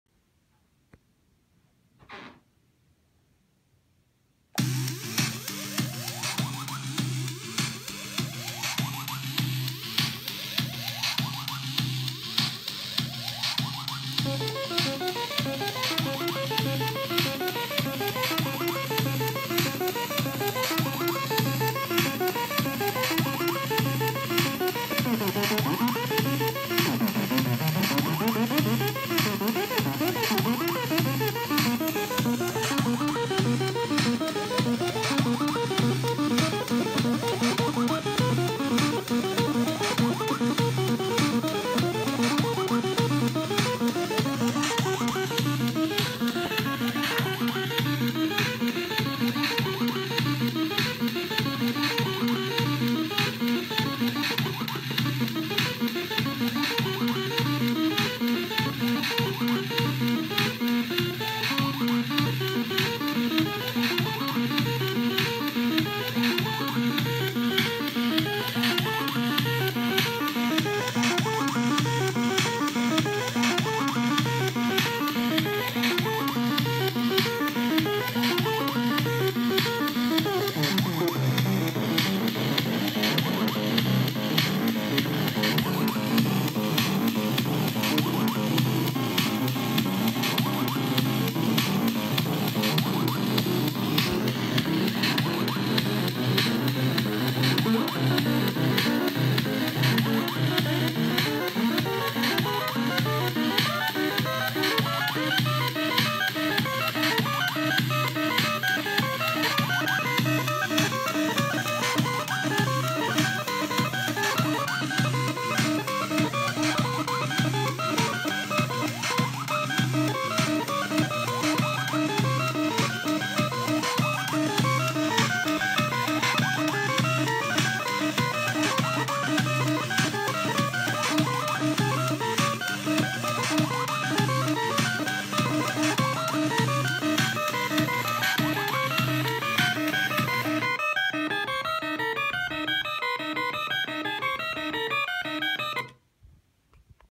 Synced up the SparkPunk with the Korg Sample and driving the clock from an ipad app
the ipad connection was more trouble than it was worth